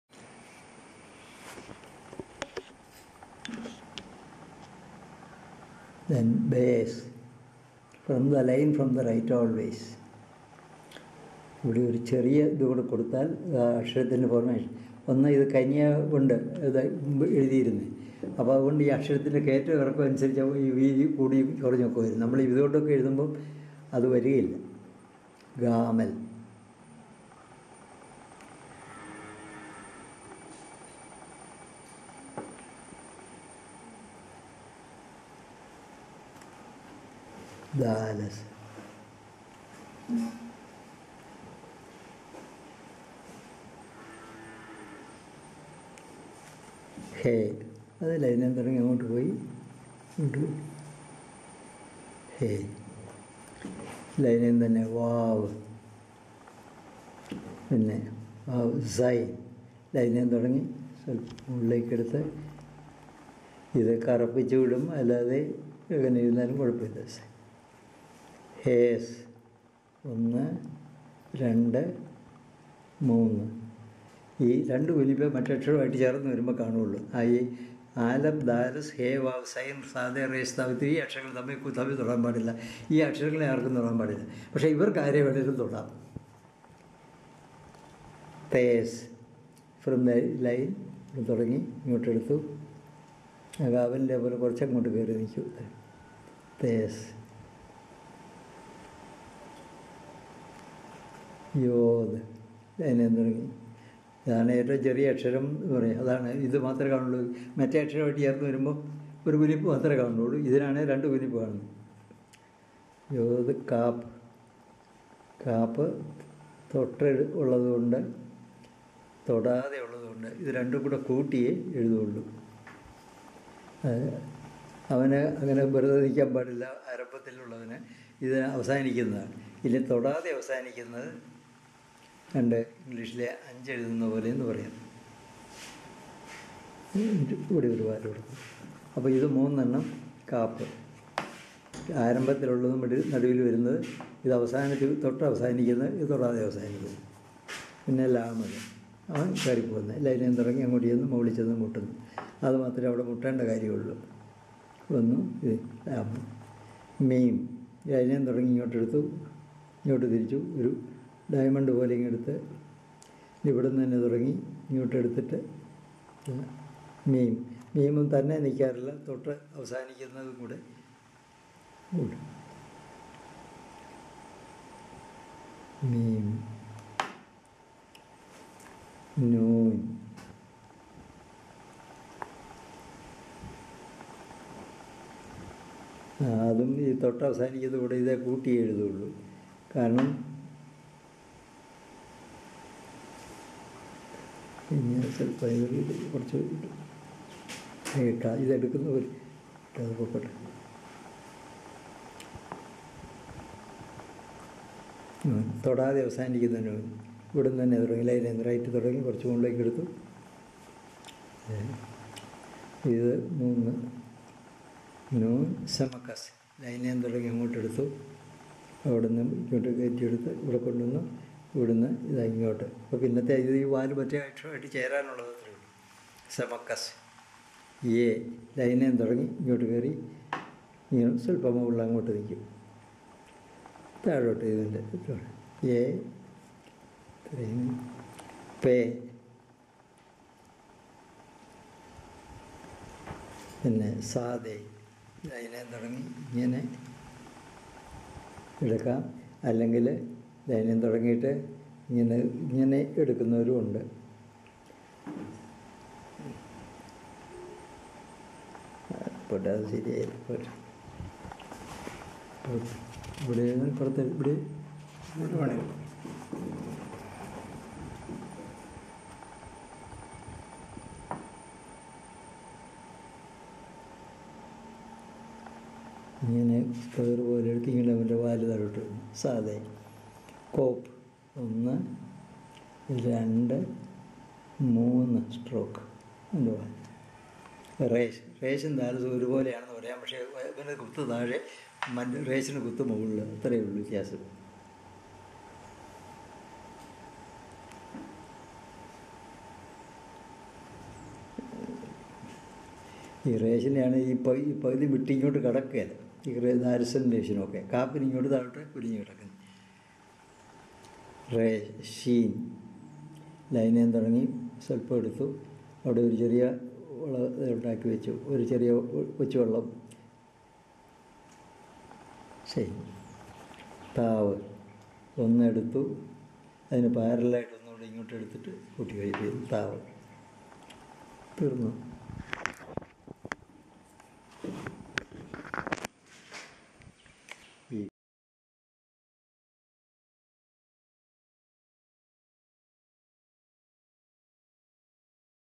Syriac Lessons by Fr. Emmanuel Thelly - Part 1
Venue: Beth Aprem Nazrani Dayra.
Opportunity to Learn East Syriac at the Beth Aprem Nazrani Dayra, Kappumthala, Kottayam.